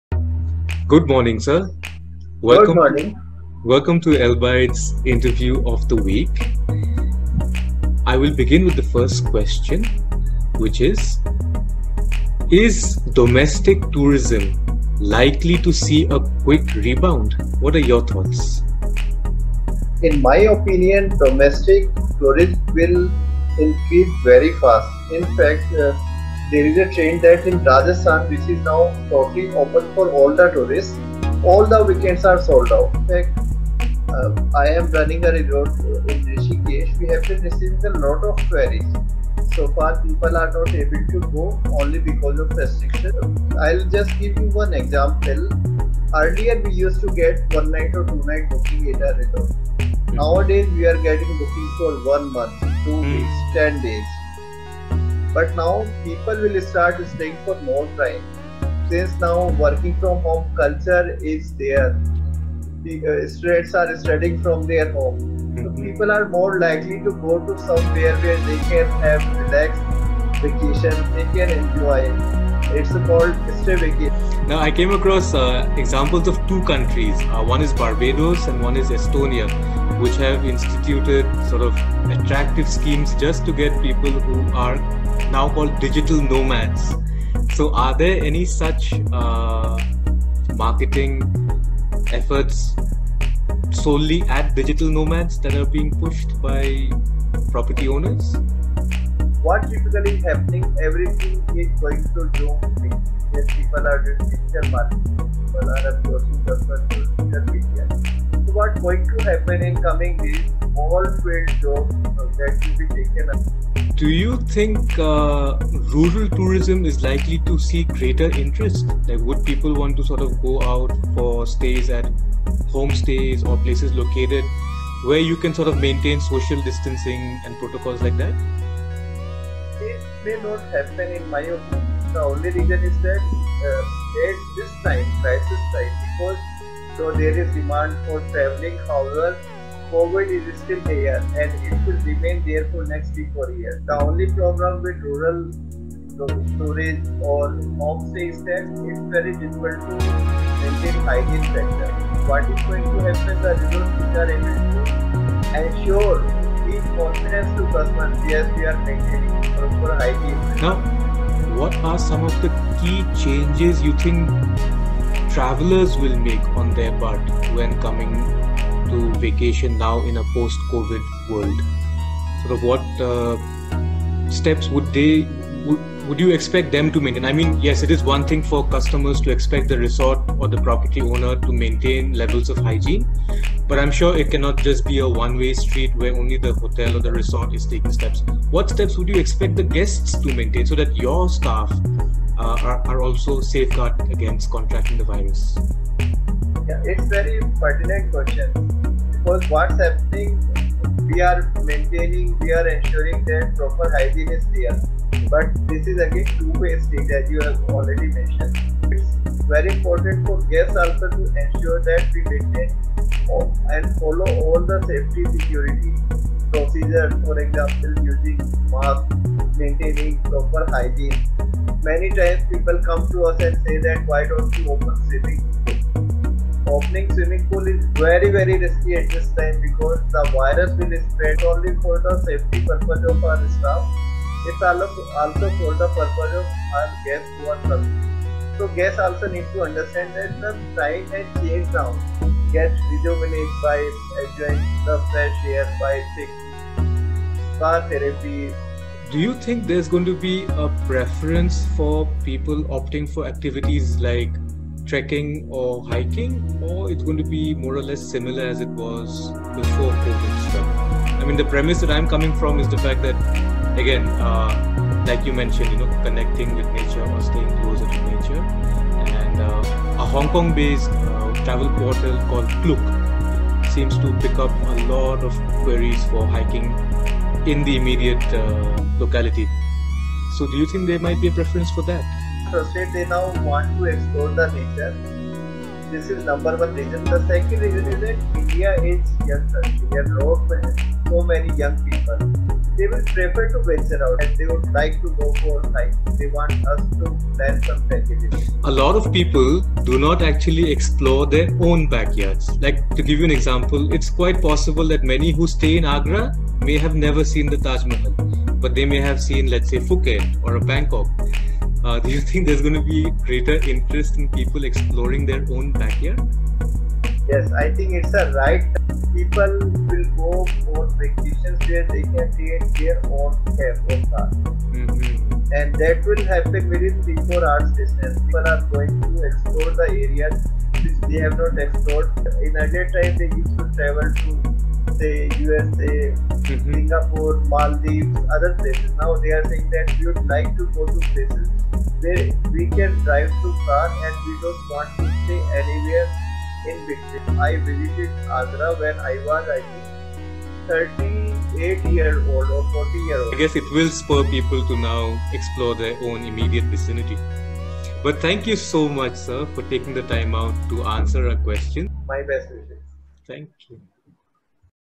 Post Pandemic Travel Trends
This is Elbyte's second interview and our guest is Neeraj Gupta, Managing Director at Panambi Vacations Pvt. Ltd. He is a passionate travel industry entrepreneur with resorts in Rishikesh, Uttarakhand.